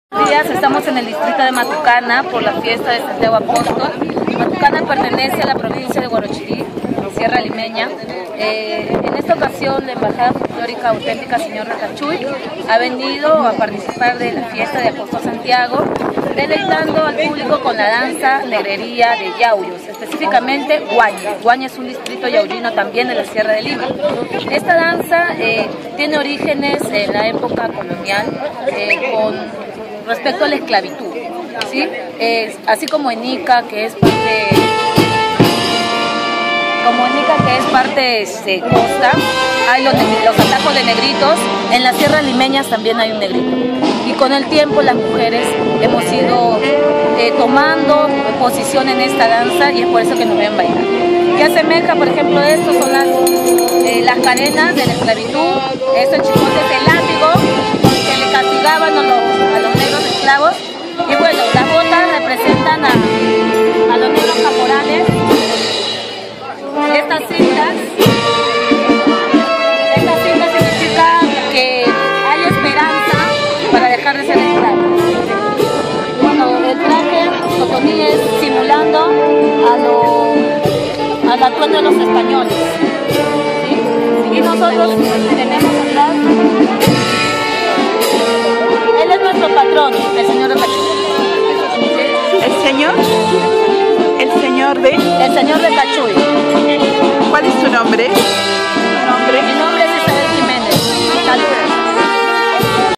Las autenticas Señor de Cachuy, entrevista Isabel Jiménez Matucana 2018.
Tras la presentación de en la ciudad de Matucana, de la Asociación Cultural "Las autenticas Señor de Cachuy", la joven miembro de la agrupación, doña Isabel Jiménez, tuvo la generosidad de conceder una entrevista para el presente canal, en el que explicó, quienes eran y detalles de la danza entre ellos, el significado del atuendo de la danza, de los Negritos de "Huañec  de Yauyos".
Isabel-